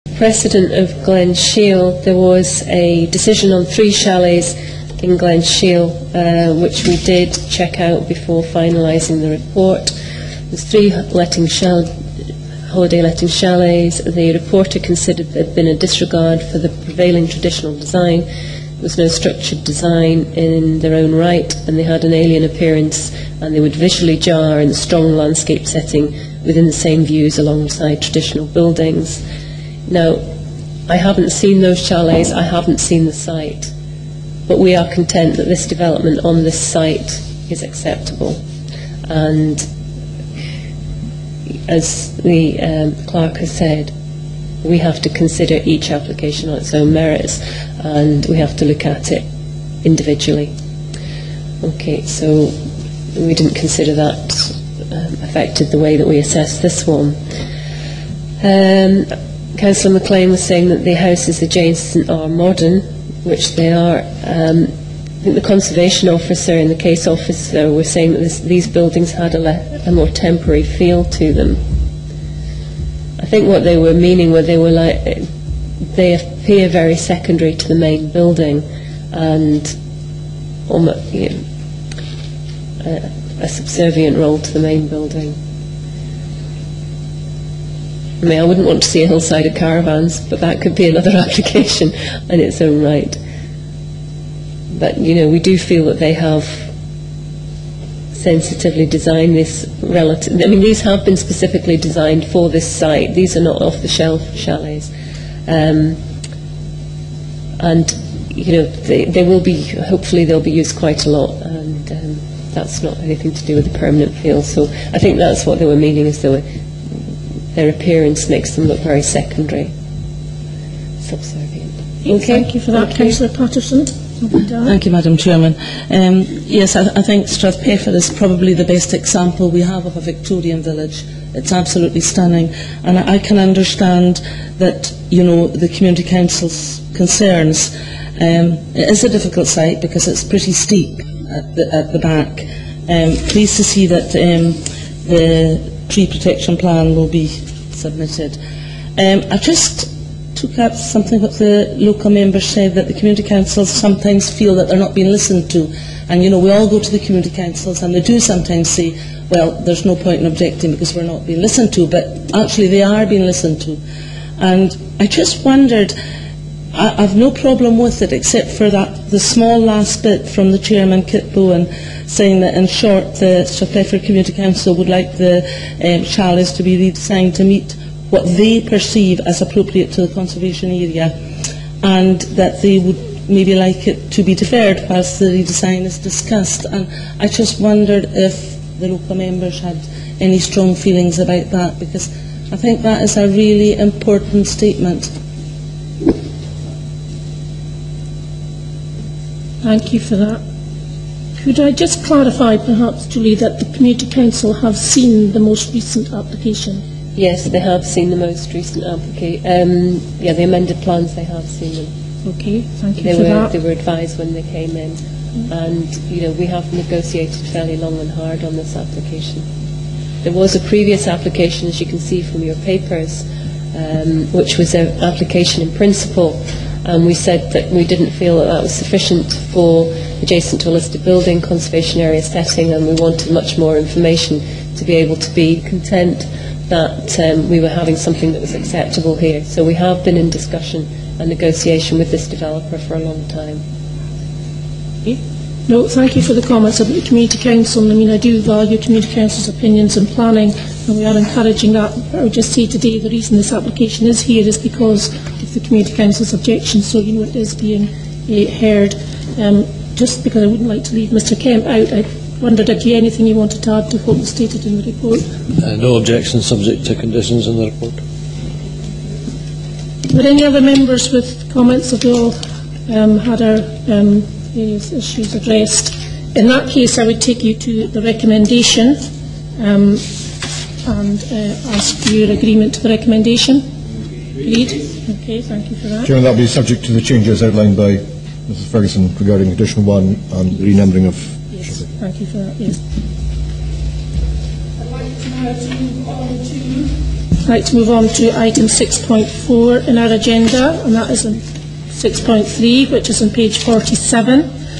Strathpeffer Planning 33 Part 4
Part of the webcast from the Highland Council Planning Committee meeting on 19 June 2012. The application on this video was for the building of a manager's house and 7 chalets behind the Ben Wyvis Hotel in Strathpeffer. The Community Council had objected to the design but the application was granted.